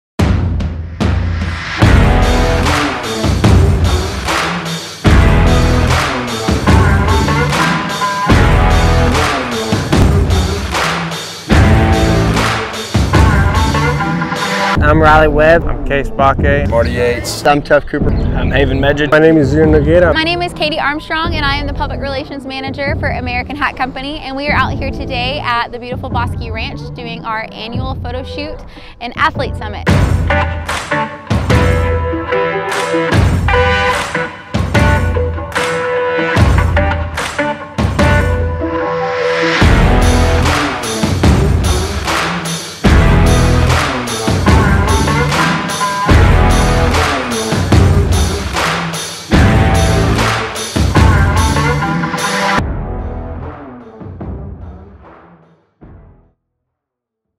Happenings at Bosque Ranch: American Hat Photoshoot
Our good friends at American Hat Company spent the week at Bosque Ranch for their yearly photoshoot!